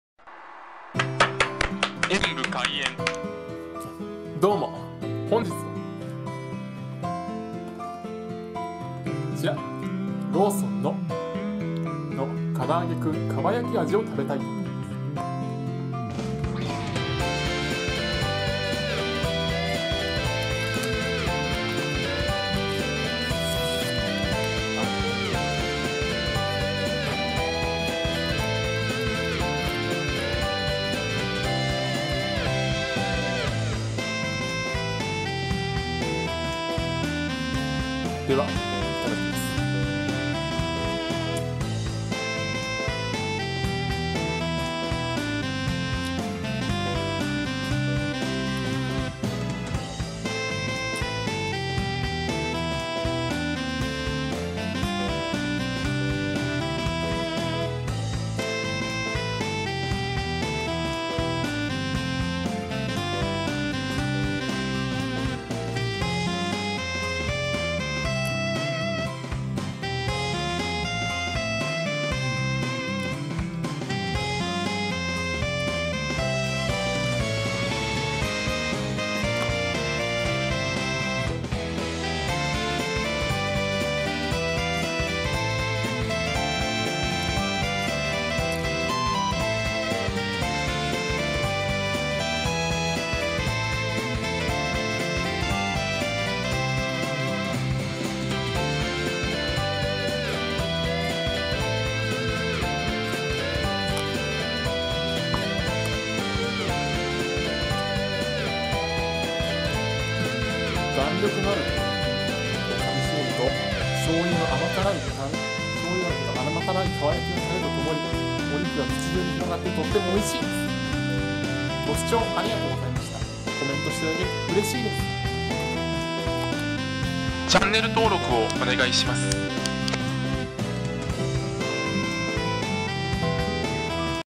ローソン 唐揚げ君蒲焼き風を食べてみた
ローソン　唐揚げ君蒲焼き風を食べてみました。
只の唐揚げ君よりも醤油が香ばしくて食欲をそそられます！！